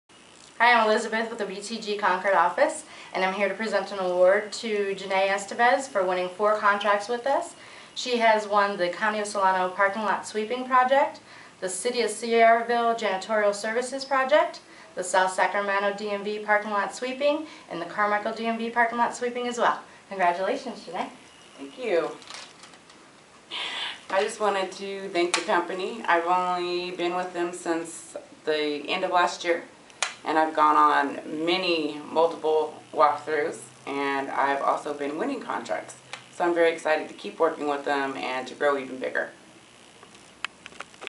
Biz-Gov: Business Thru Government Hall of Fame JDR Building Construction
Biz-Gov client inducted into the Hall of Fame